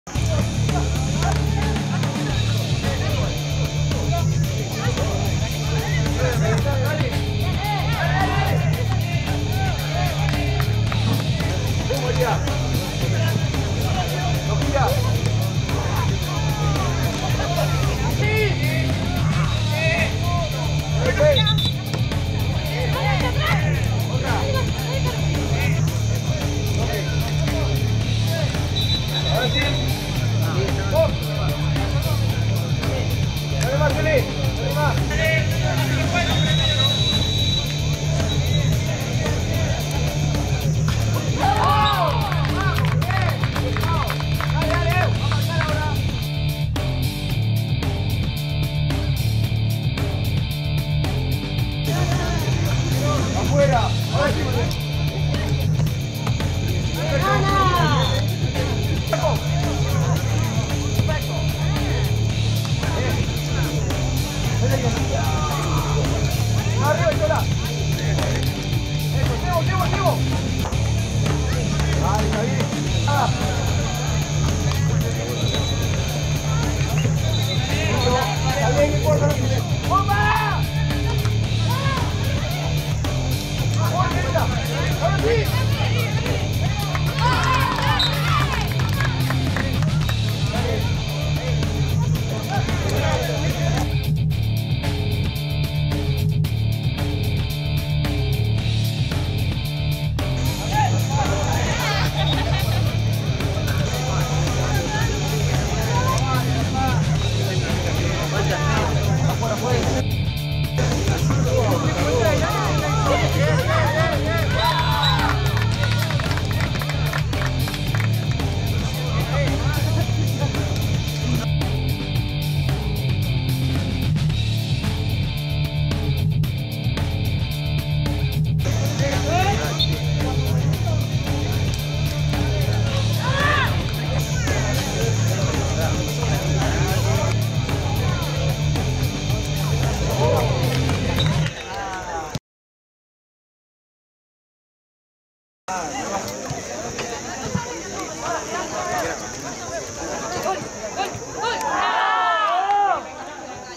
MiráCup | Fútbol Femenino | Espartanas vs La Re Junta (Carhué)
Algunas jugadas y goles de uno de los partidos femeninos de nuestro torneo de fútbol en Coronel Suárez.